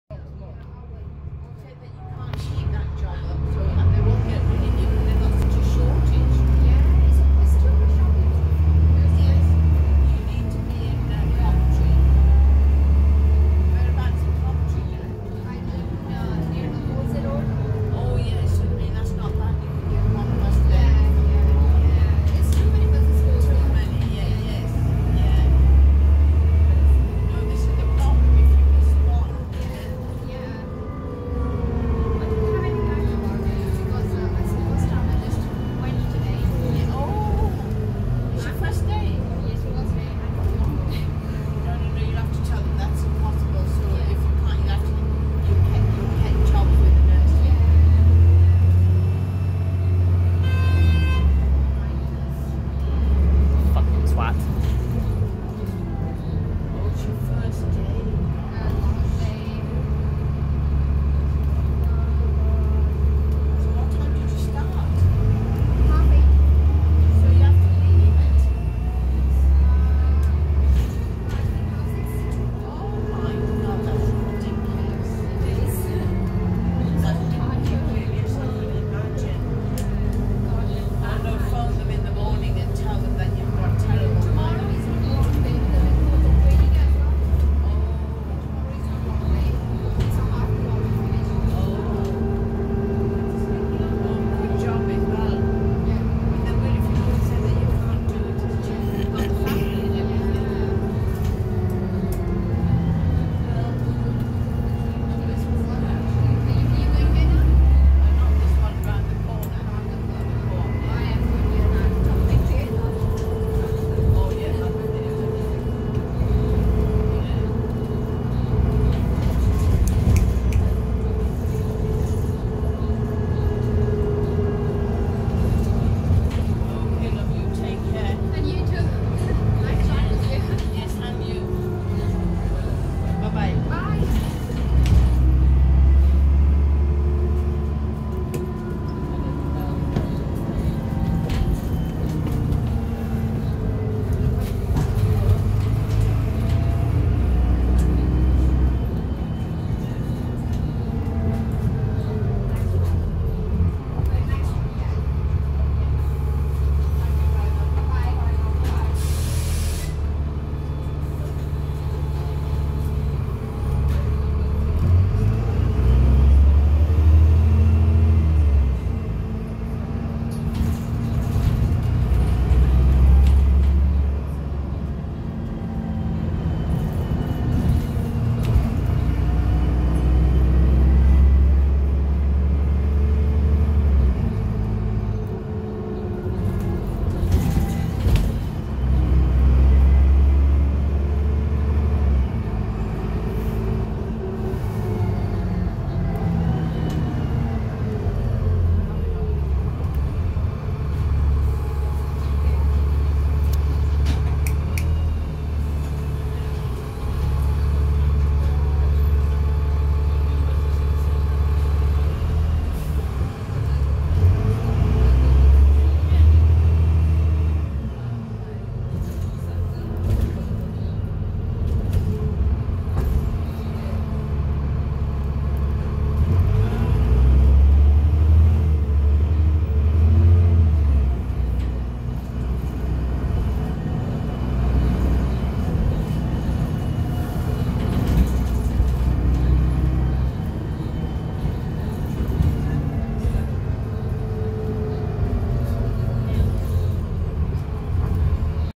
Stagecoach Midlands 18398 KX55 TLY
Here we see a journey on Transbus/ALX400 Trident 18398 KX55 TLY seen on route X17 to Coventry.